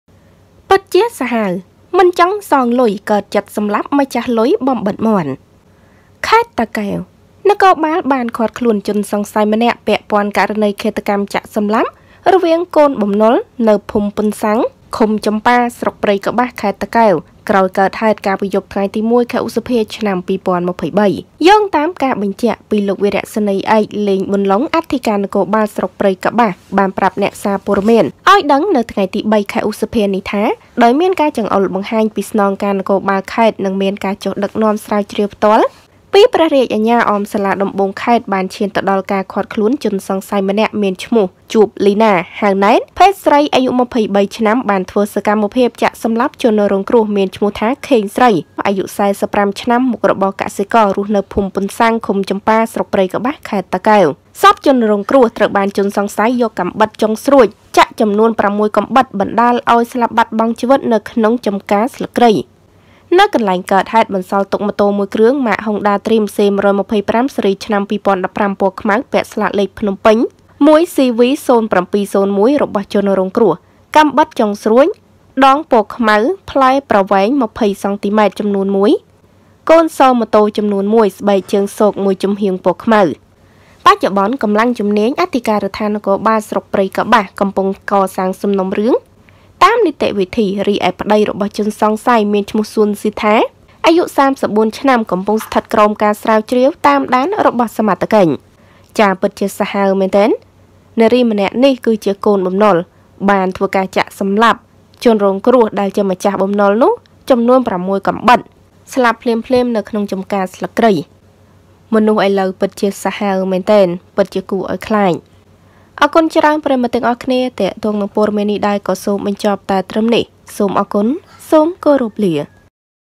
សាហាវ! មិនចង់សងលុយ កើតចិត្តស.ម្លាប់ម្ចាស់លុយបំបិទមាត់
សូមអធ្យាស្រ័យ

ចំពោះការមិនបានតប ខំមិនបានគ្រប់គ្នា

Ms Socheata